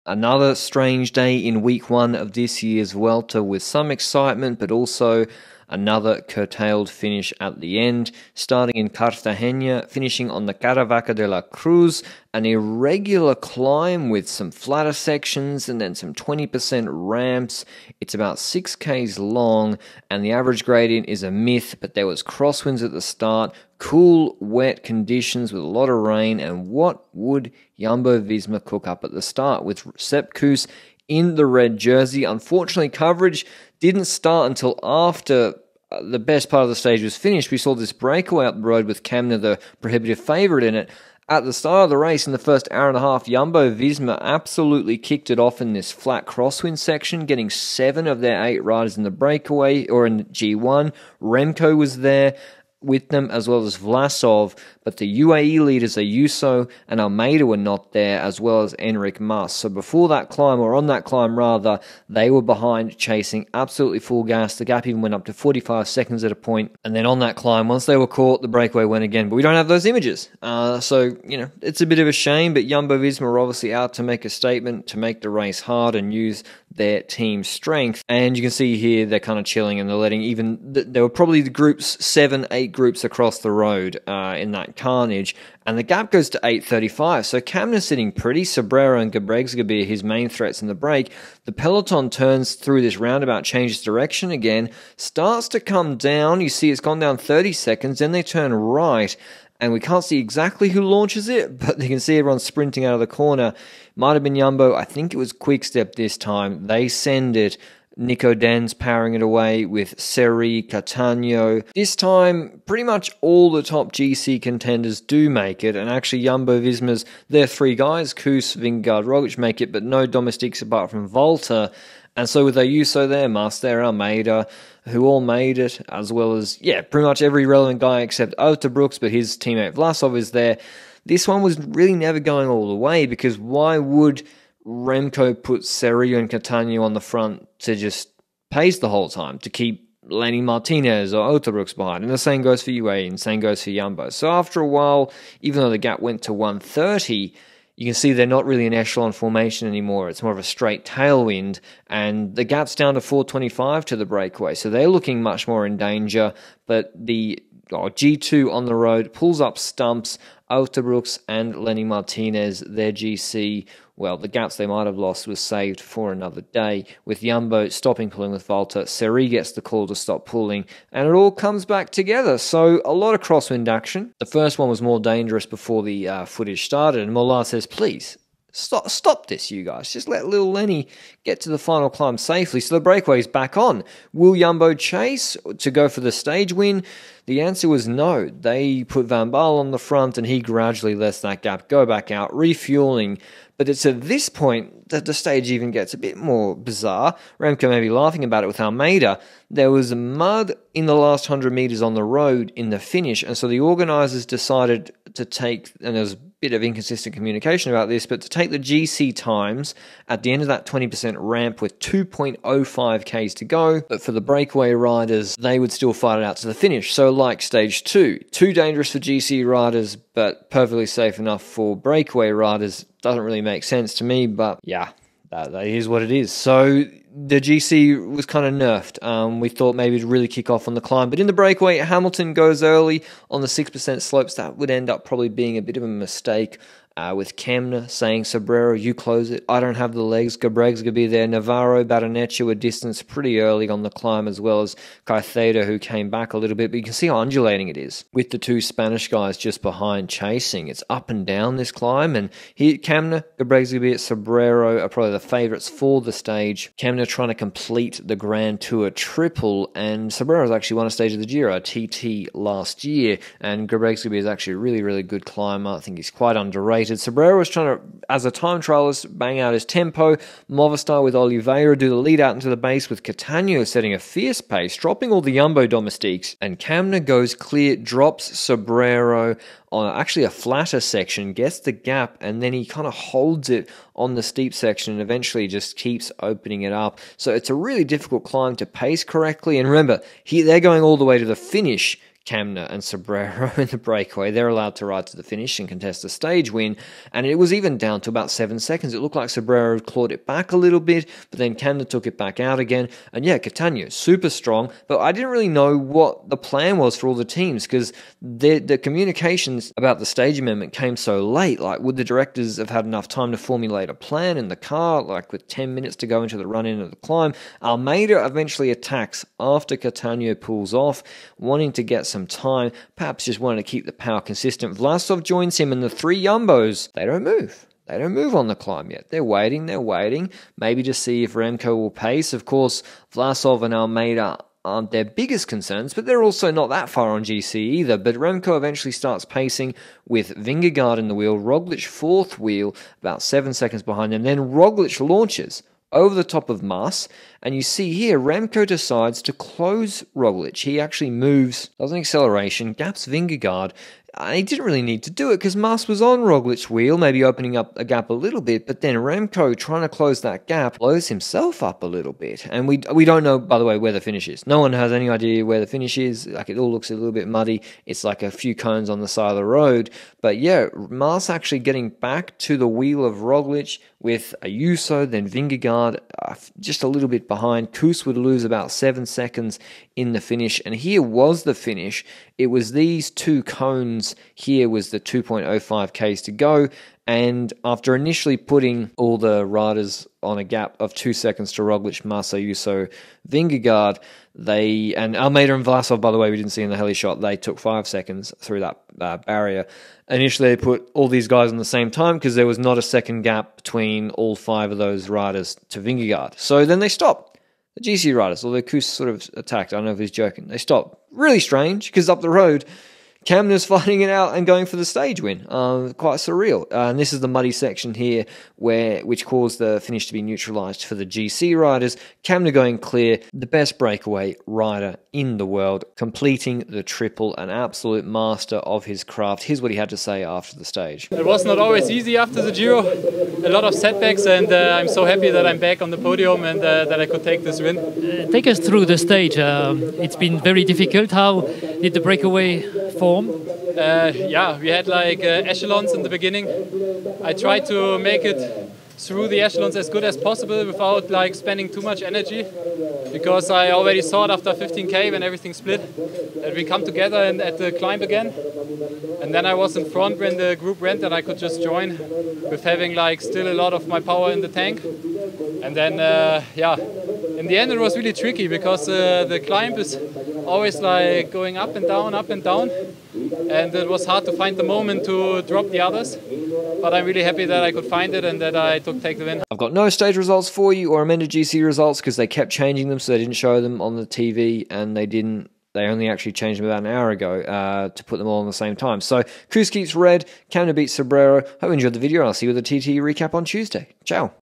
Your Face When Remco Says the Stage Finish Doesn't Exist | Vuelta a Espana 2023 Stage 9
Lanterne Rouge presents highlights of Vuelta a Espana 2023 Stage 9.

Credits:
Footage | Amaury Sport Organisation
Photos | Cor Vos (unless stated otherwise)